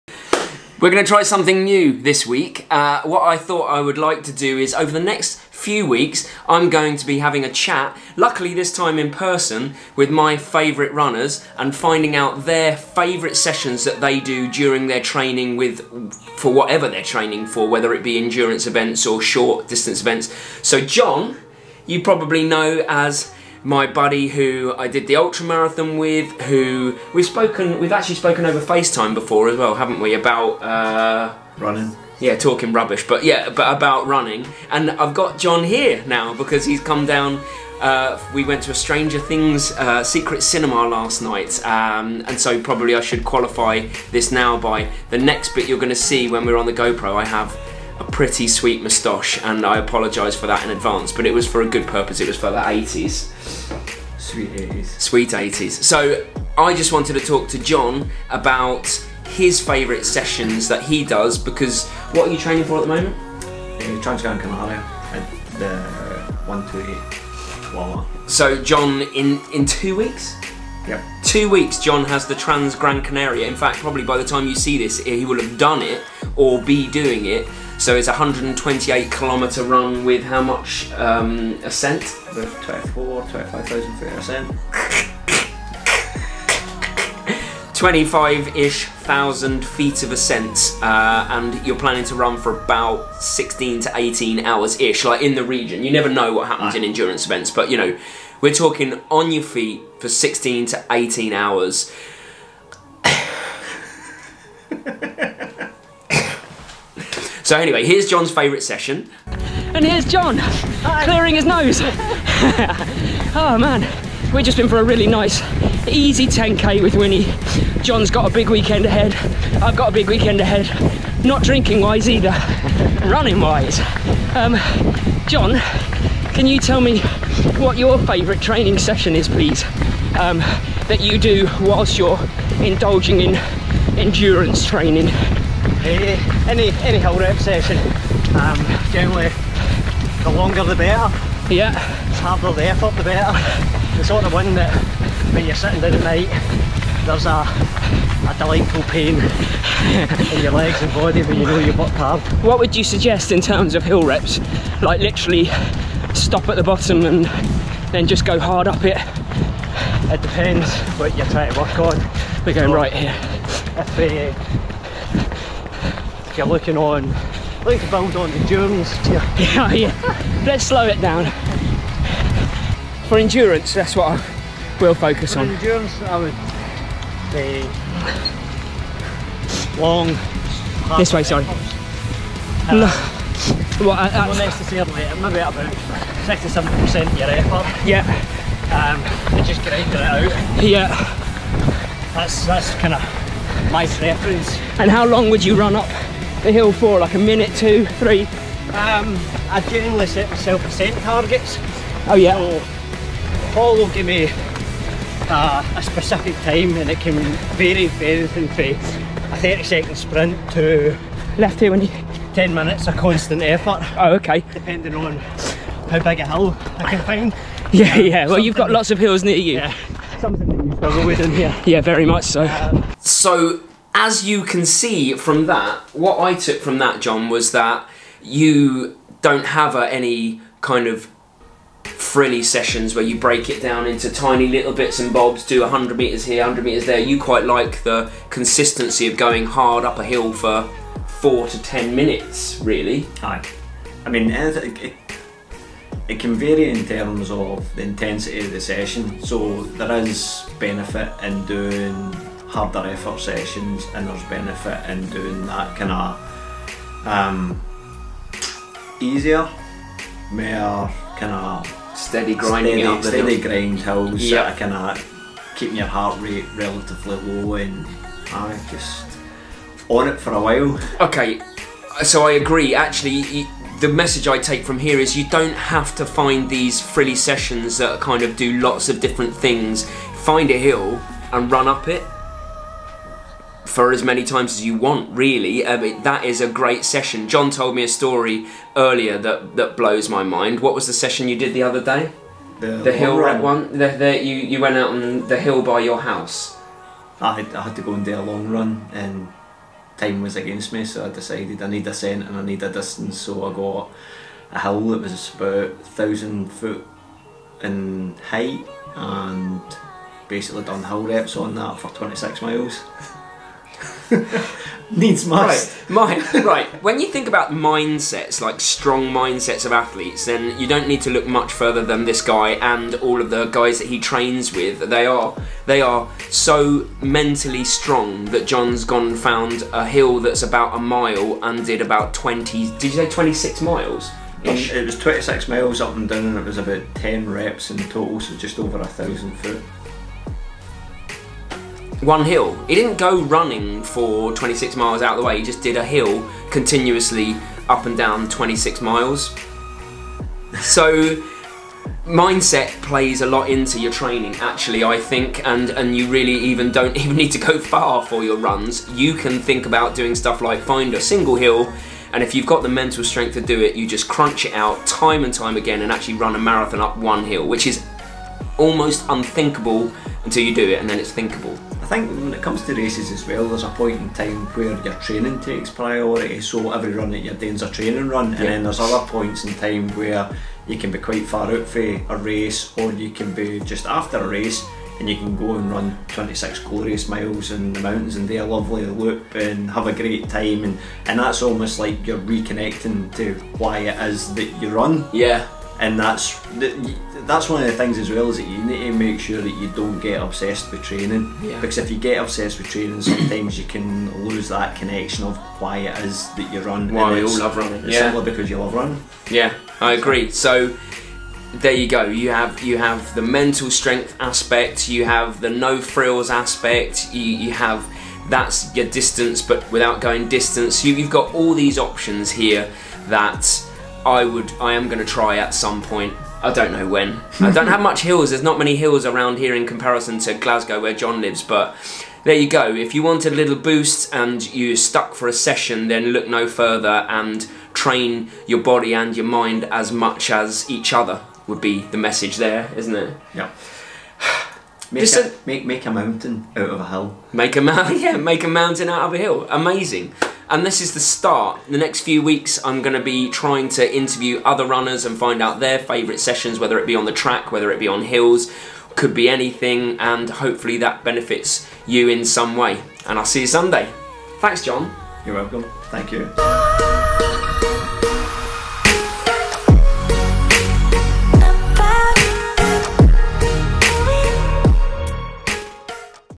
My favourite runner's favourite sessions - John Connolly
This week on my favourite runner's favourite sessions I have a chat (and a run) with top amateur ultra runner and one of my best buddies, John Connolly. I was keen to learn about his mindset for training and the sessions he likes to do.

Be warned, there is no frilly session with John. It's just good old fashioned hard graft. Just how we like it!

If you like these types of video then feedback is good and I can make some more.